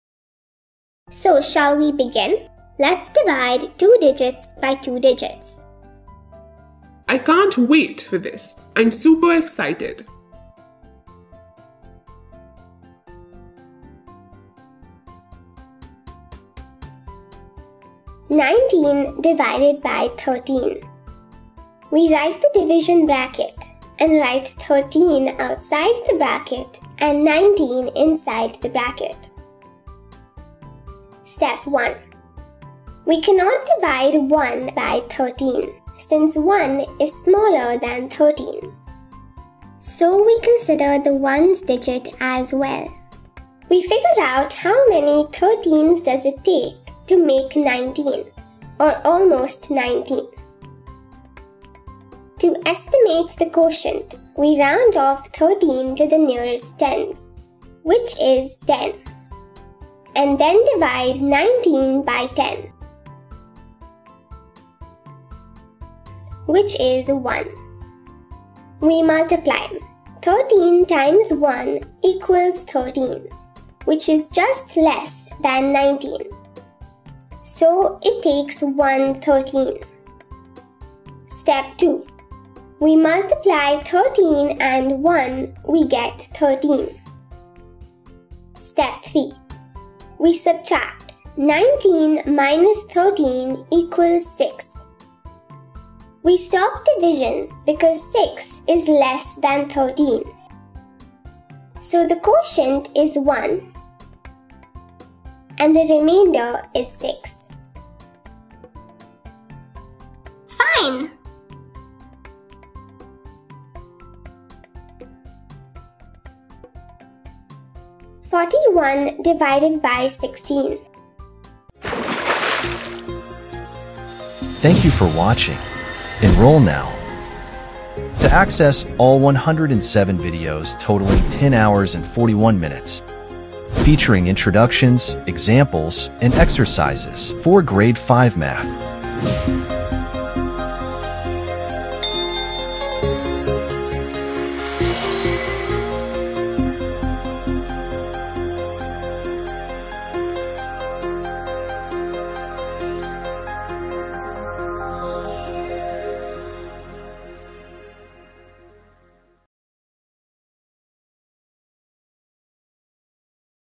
G5 - Module 5- Exercise 1- Divide 2 Digit by 2 Digit - Level 1 | Appu Series | Grade 5 Math Learning
Ready to tackle two-digit division? Appu and Tiger are here to make it super easy! Start with simple problems and watch your skills grow as you go. It’s like solving a mini puzzle - let's plunge in and have some fun while we divide!